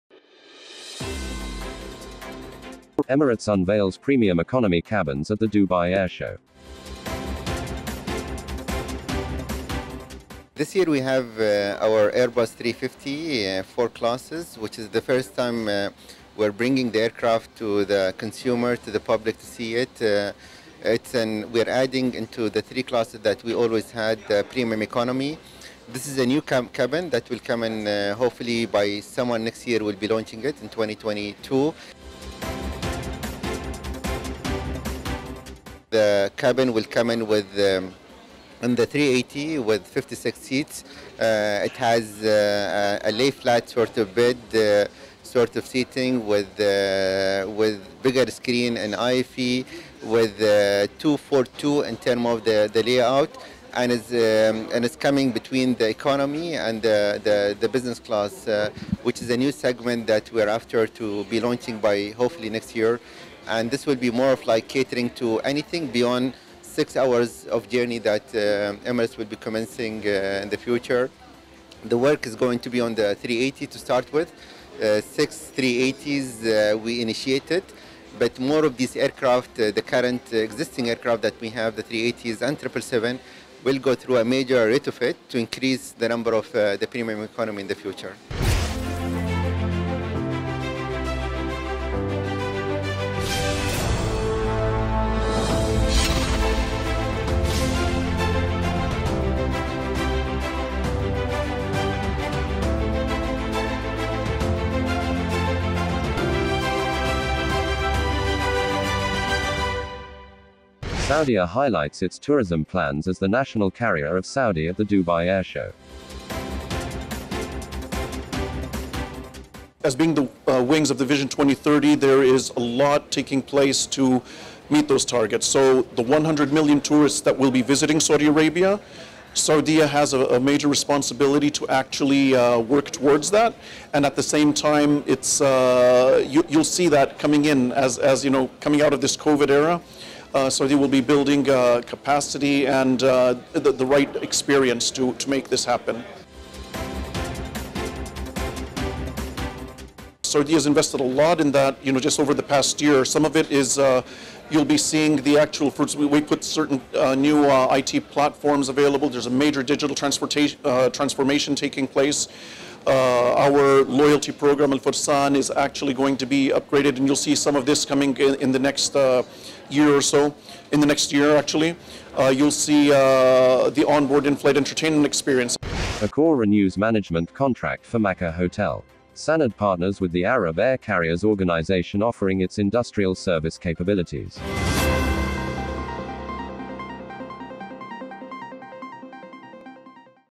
Middle East Episode 382 (Nov 21, 2021)
- In this episode TravelTVMiddleeast.News focuses on:

- Emirates unveils Premium Economy cabins at the Dubai Airshow
- Saudia highlights its tourism plans as the national carrier of Saudi at the Dubai Airshow
- Accor renews management contract for Makkah hotel
- Sanad partners with the Arab Air Carriers Organization offering its industrial service capabilities

- We Interviewed:

- ADNAN KAZEM, Chief Commercial Officer, Emirate Airlines
- CAPTAIN IBRAHIM S KOSHY, Chief Executive Officer, Saudia